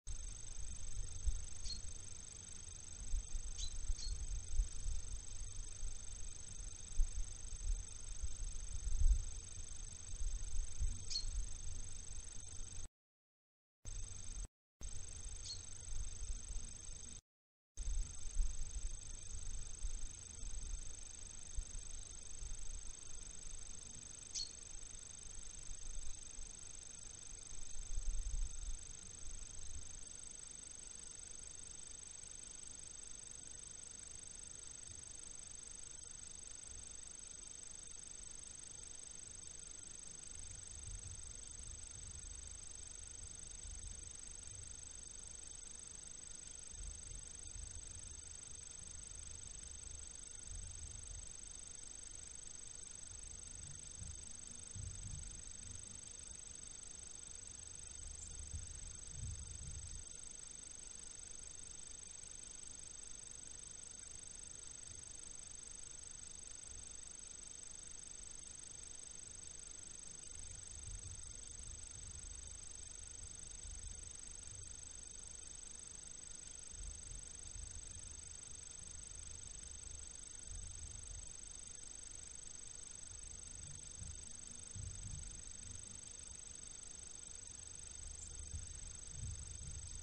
Territorio Lince. Comparativa de búhos
Vídeo comparativo de búhos vistos en el canal wwftv propiedad de WWF a través de su iniciativa Territorio Lince: